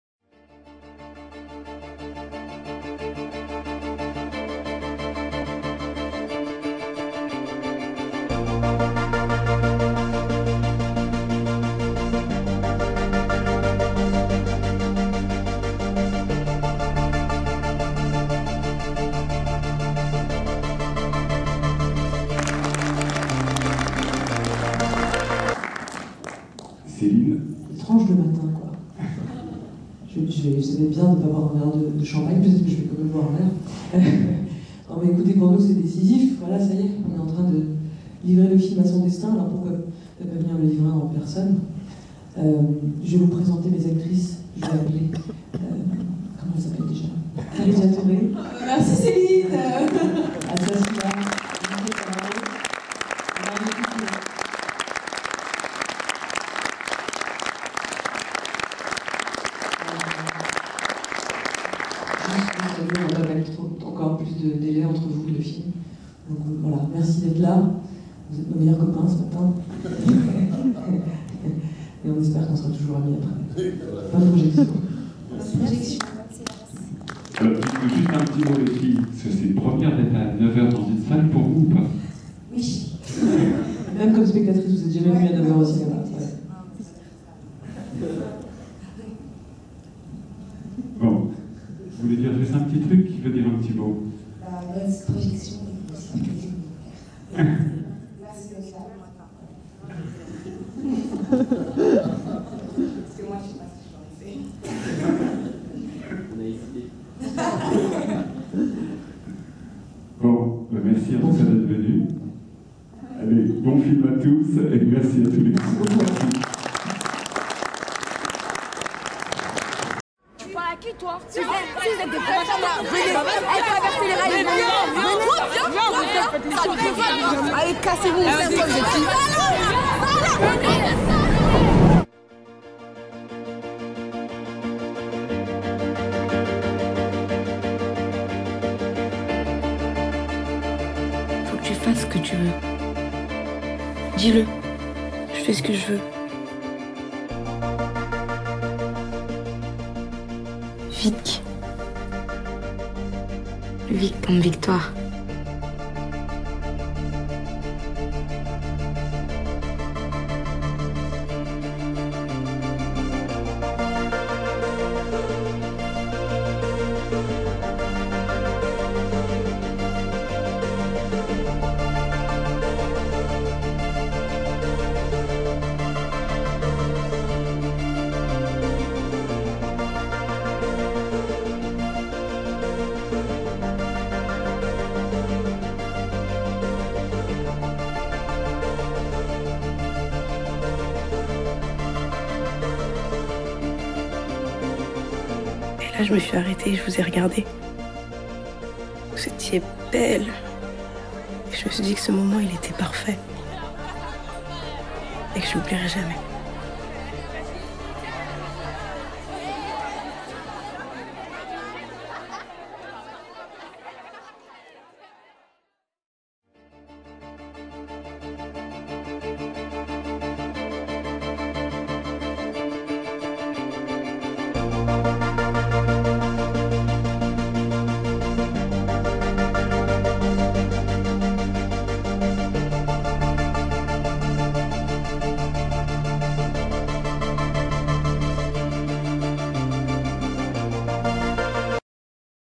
Avant-Première du film "Bande De Filles" avec Karidja Touré
Voici une vidéo de l'avant-première du film "Bande De Filles" qui s'est déroulé le 22/10, enjoy :-)

Vous pouvez retrouver mon article sur le film "Bande de Filles"  en cliquant sur le lien